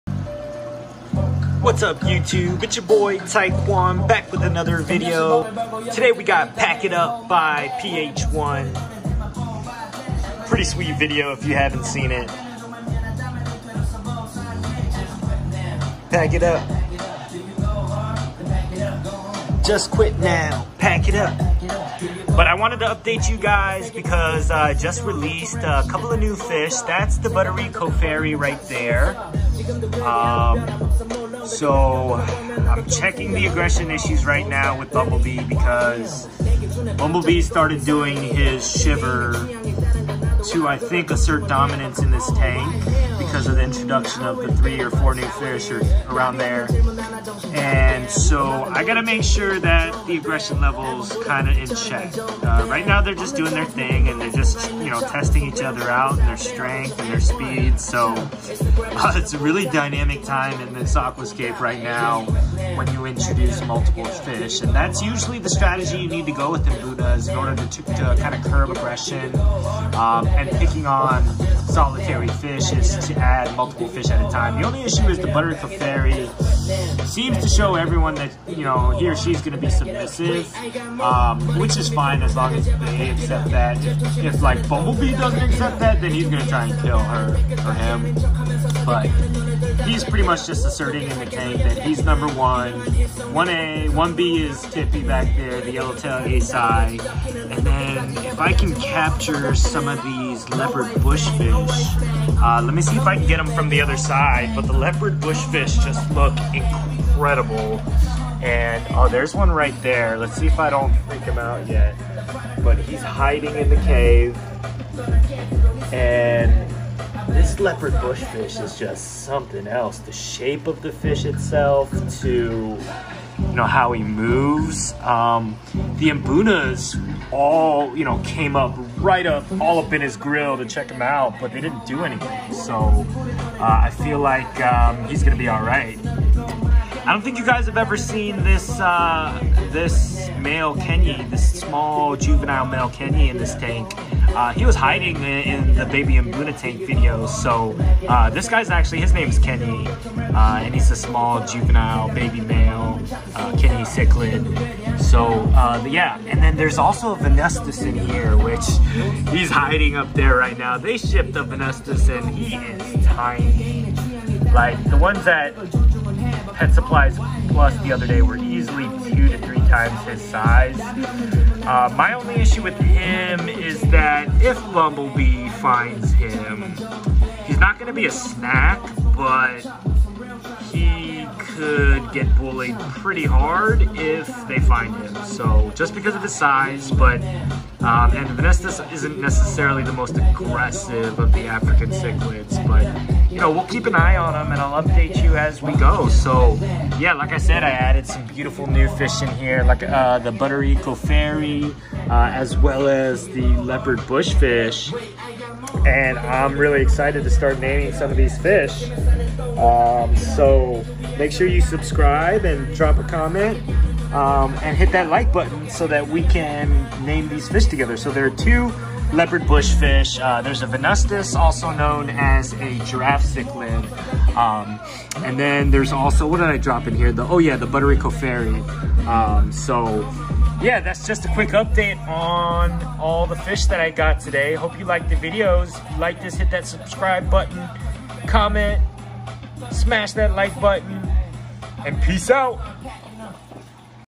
*RELEASING NEW* Leopard Bushfish, Buttikoferi Tilapia & Venustus into BABY MBUNA TANK in 4K!!! 🐠🐠🐠
Check out the release of my new Leopard Bushfish, Buttikoferi and Venustus Cichlids into their new habitat!!!